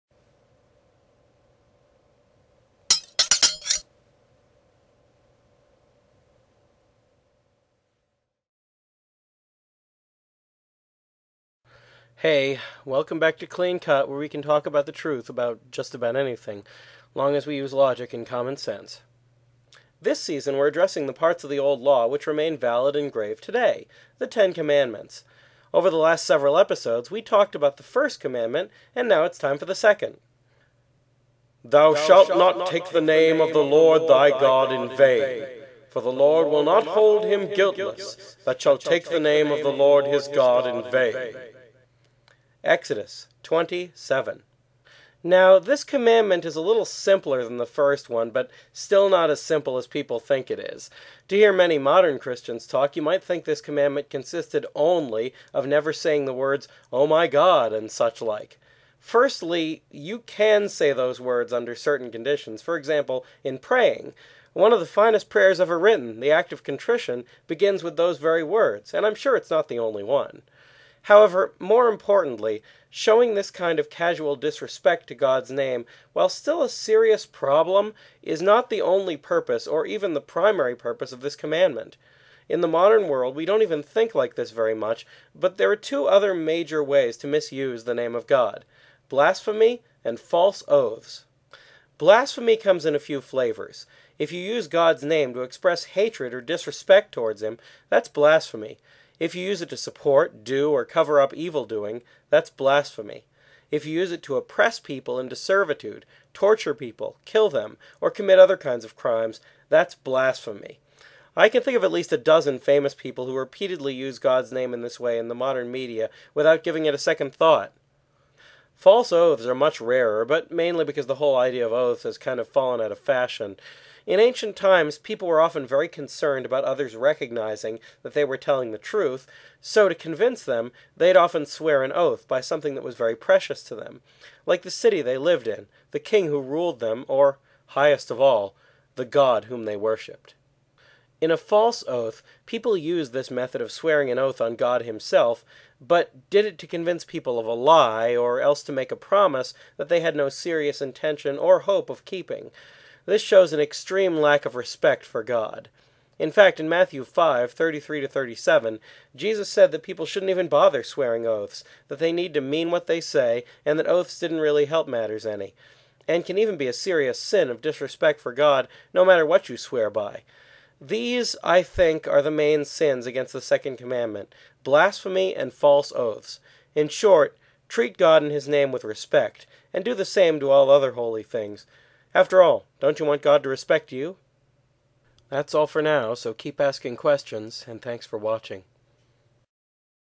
How Does One Take God's Name in Vain?
What does it mean to take God's name in vain?  Is it more than just casual disrespect?  What's blasphemy?  What are false oaths?